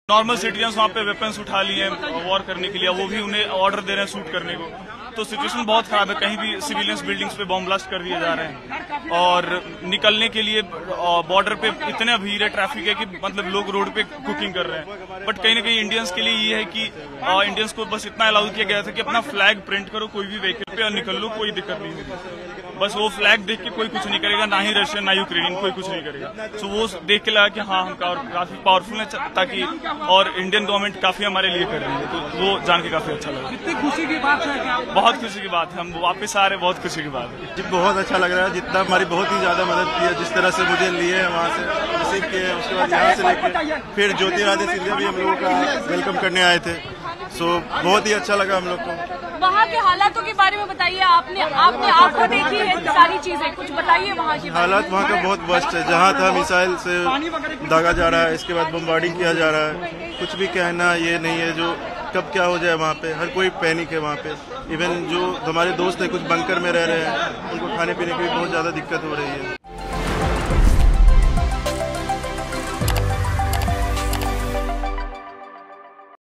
Student Evacuated from Ukraine: यूक्रेन-रूस हमले के बीच भारत लौटे छात्र, बताई दहशत की सारी कहानी
यूक्रेन और रूस के बीच जारी जंग के बीच भारत अपने नागरिकों को बचाने में जुट गया है। छात्रों का जत्‍था वतन पहुंच गया। वतन और घर वापसी की खुशी छात्रों के चेहरे पर देखी जा सकती है। छात्रों ने बताया कि वहां की स्थिति काफी खराब हो चुकी है। वहां के नागरिकों ने हथियार उठा लिए हैं। पूरे देश में जंंग का माहौल बना हुआ है। देखिए क्या कहा छात्रों ने।